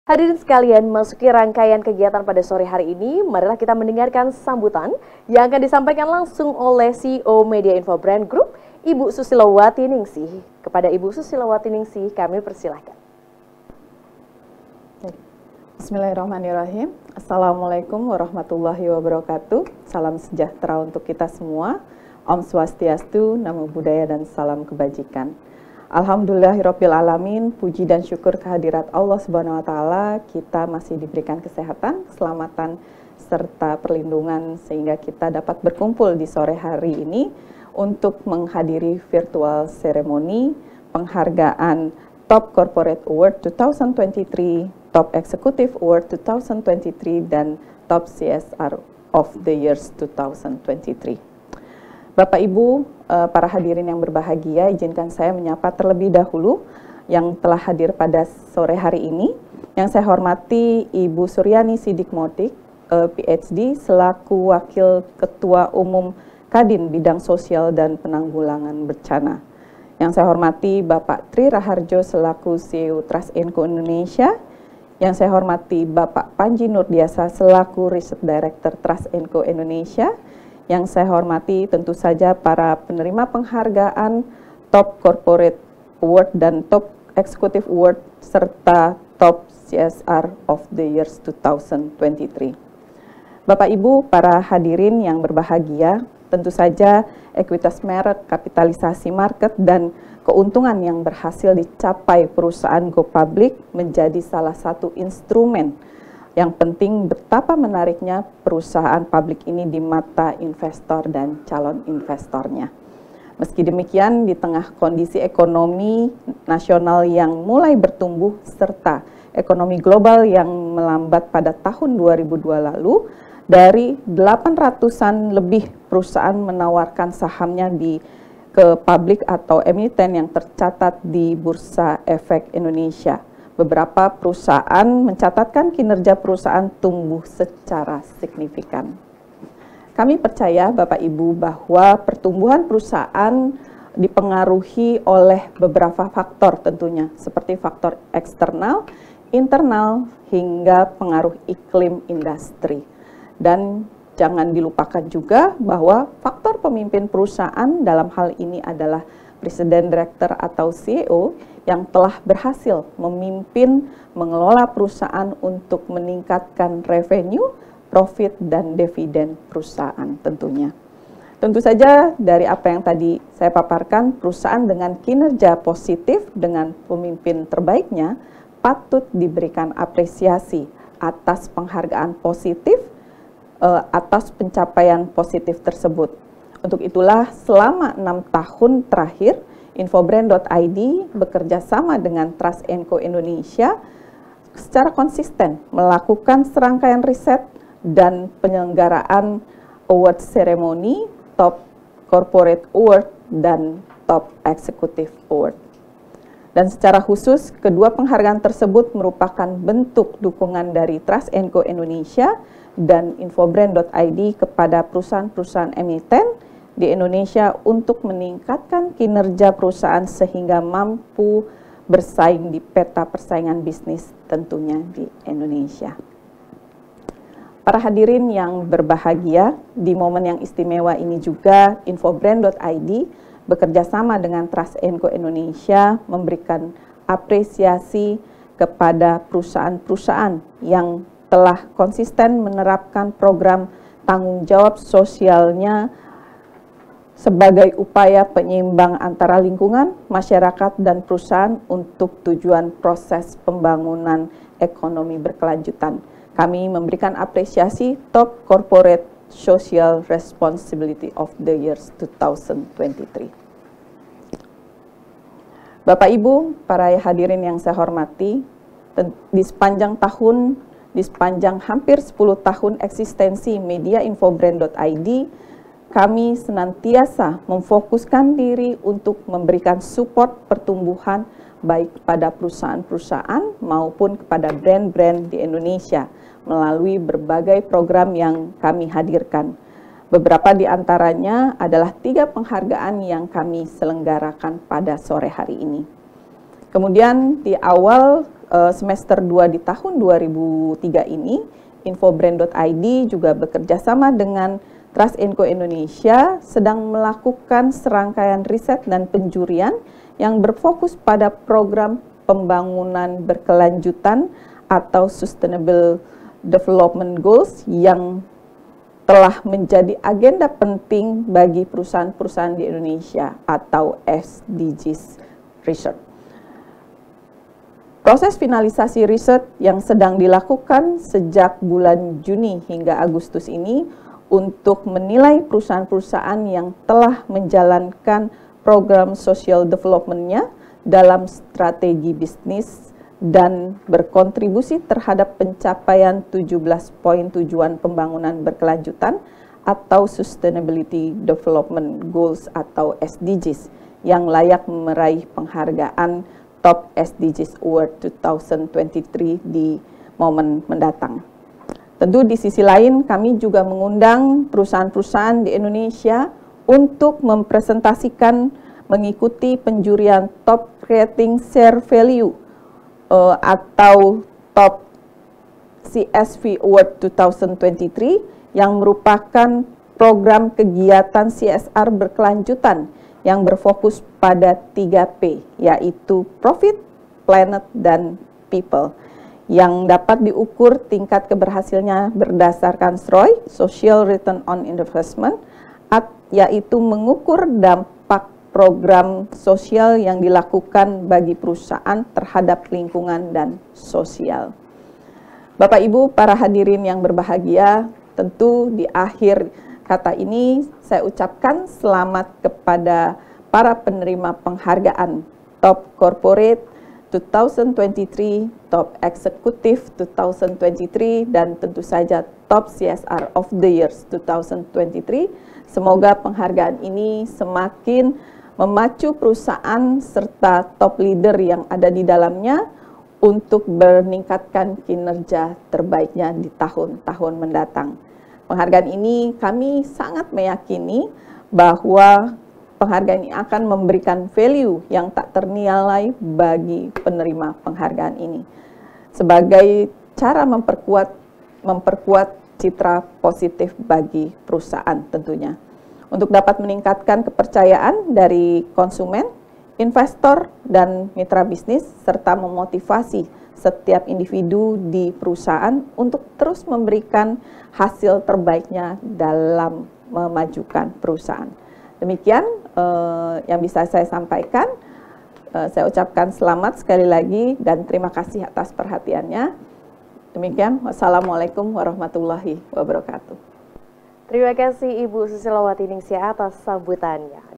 Sambutan CEO Media Infobrand Group dalam Pelaksanaan Virtual Award Ceremony 2023
Sebagai bentuk penghargaan terhadap perusahaaan-perusahaan yang mampu menorehkan kinerja baiknya, INFOBRAND.ID secara khusus mempersembahkan penghargaan Top Executive Award 2023.

Sebagai bentuk penghargaan bagi para pemimpin perusahaan dan juga  perusahaan perusahaan emiten yang telah berhasil mencatatkan angka prestasi terhadap perusahaan, infobrand.id dengan bangga mempersembahkan sebuah penghargaan bergengsi bertajuk Top Corporate Award, Top Executive Award dan Top CSR of The Year 2023